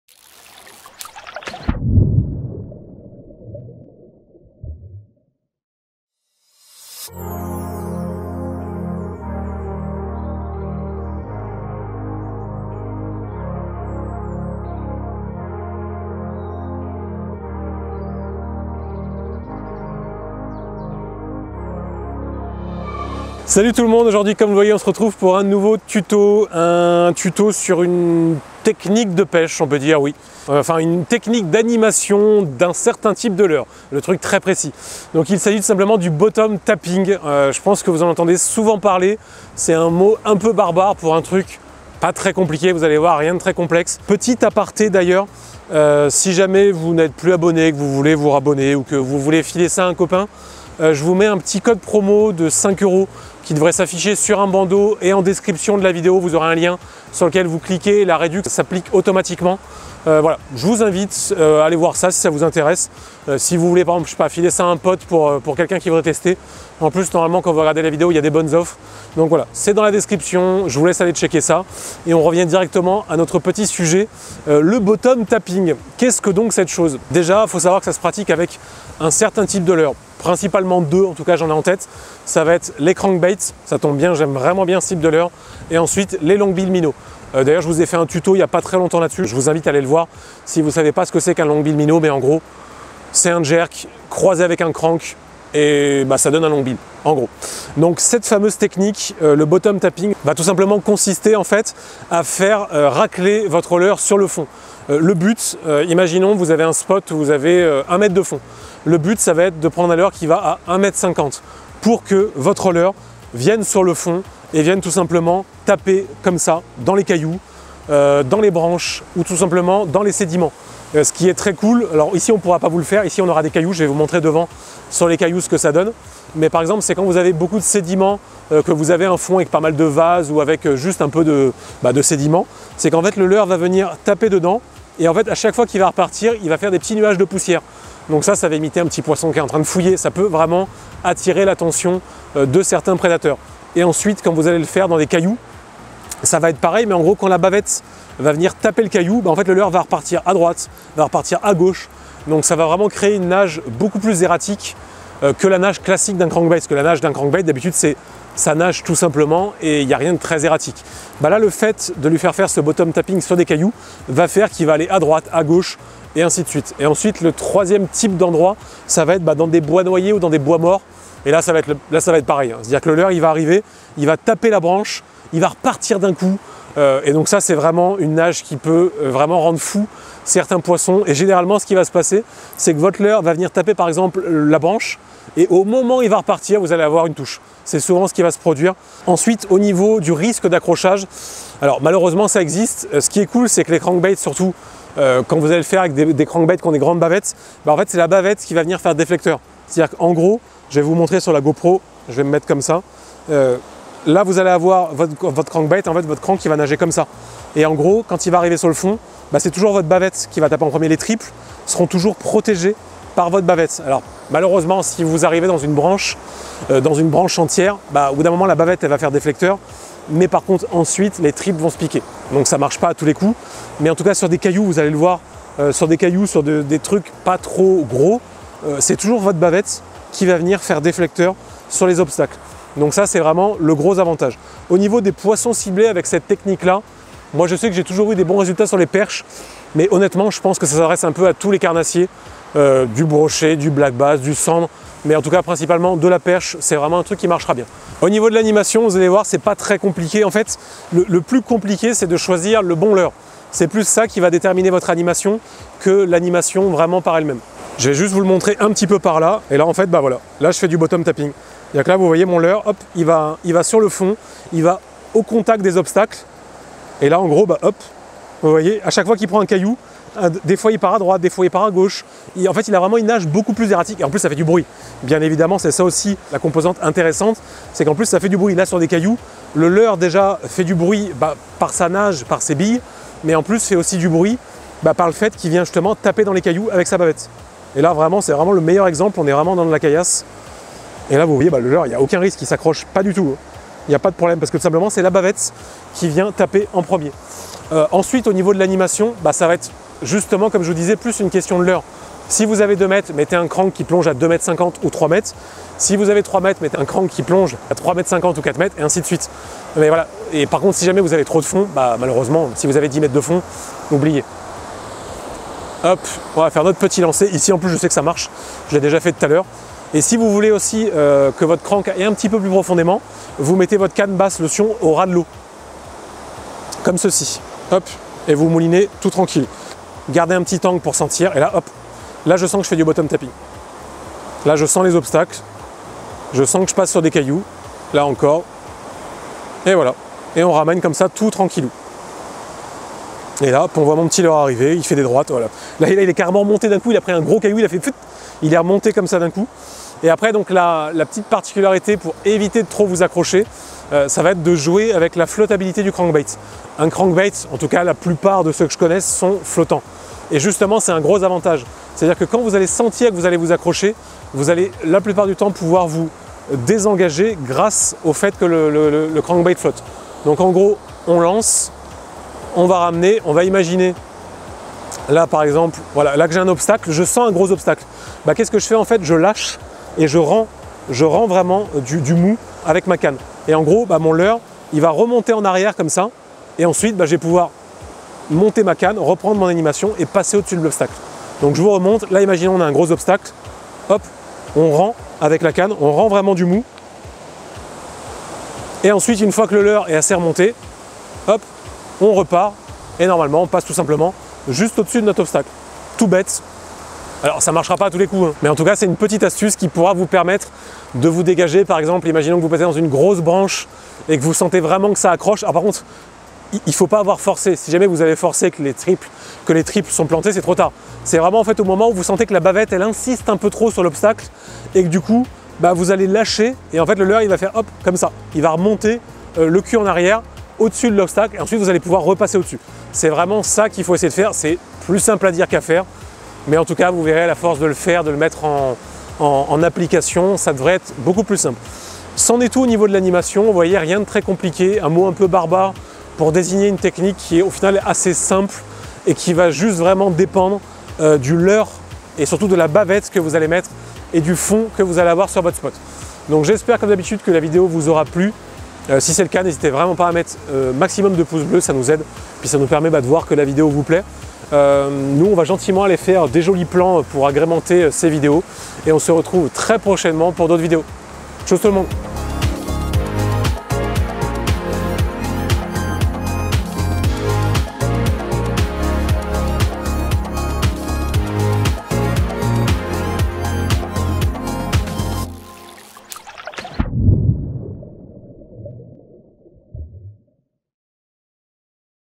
Tuto Pêche - tout savoir sur le BOTTOM TAPPING !
Dans ce nouveau Tuto pêche, je vais tenter de t'expliquer comment faire du bottom tapping avec un crankbait. Une technique de pêche intéressante pour faire bouger les perches et les autres carnnasiers.